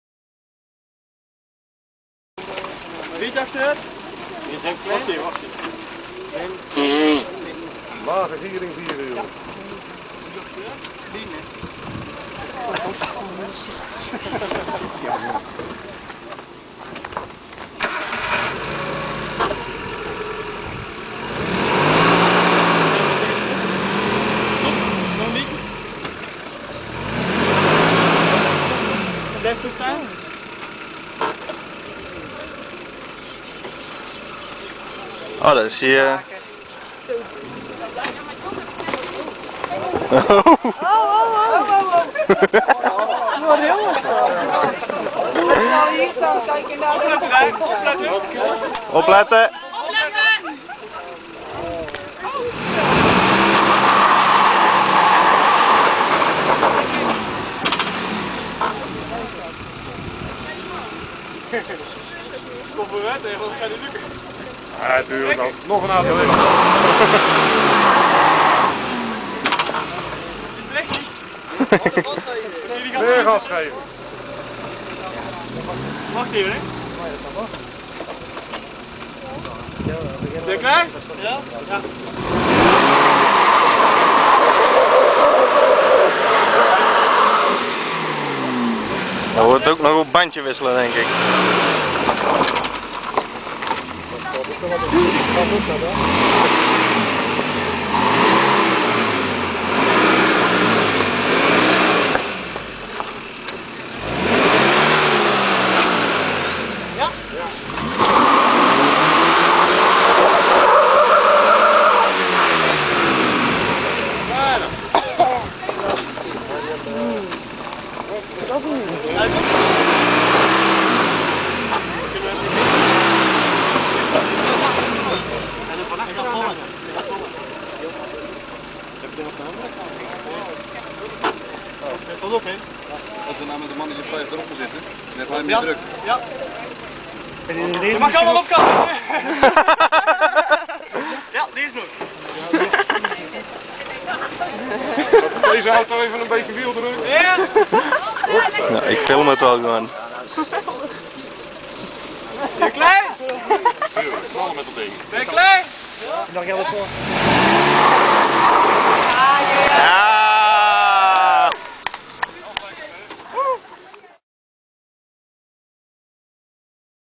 JeepSafari
We waren bijna in een ravijn gereden en moesten hier uit getrokken worden.